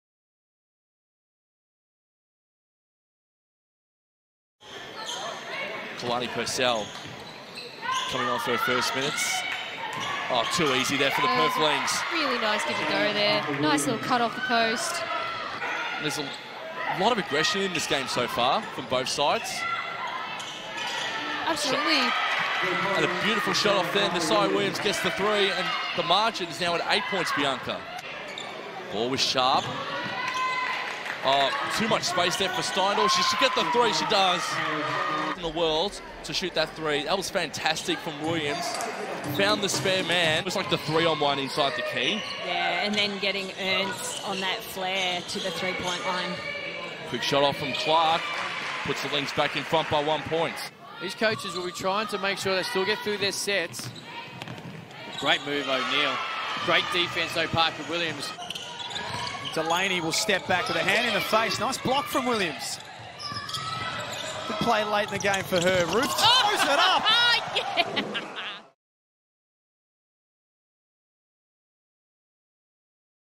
Nes'eya PARKER - WILLIAMS - WNBL20 Highlights
#️⃣3️⃣  [ Player Highlights - Nes'eya PARKER - WILLIAMS ]

"The WA junior provided a much needed bench energy adding positivity to the group. In limited time Neseya’s length showed how she could be a defensive X-Factor." Assistant Coach Keegan Crawford